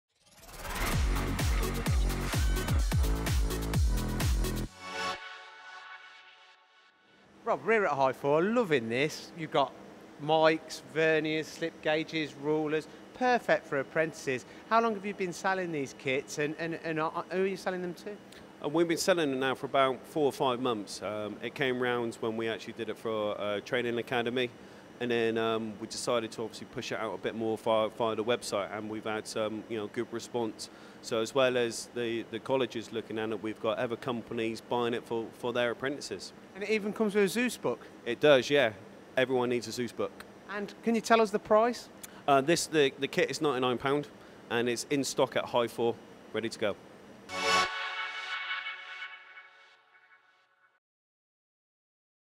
Hyfore apprentice kit just £99
A must for every trainee and apprentice. An engineers starter kit comprising of factory essentials. Micrometer's, rules, a Zeus book, feeler gauges and more. All for £99..